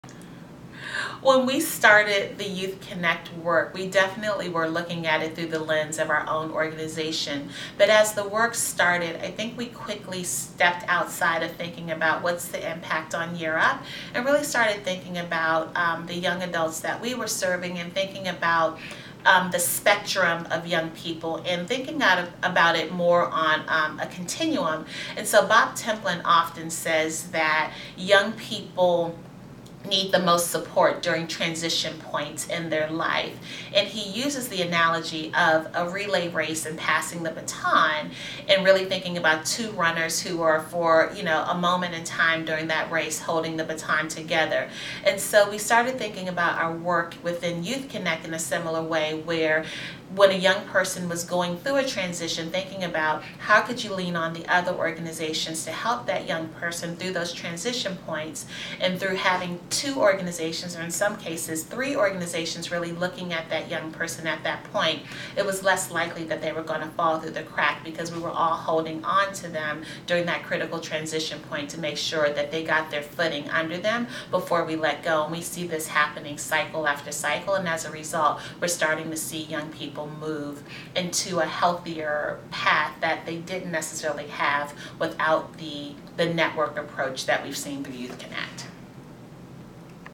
Helping Youth Through Transitions, Ronda Thompson, Year Up
Rhonda Thompson, National Director of Alumni Relations, Year Up

For information Venture Philanthropy Partners or youthCONNECT, visit our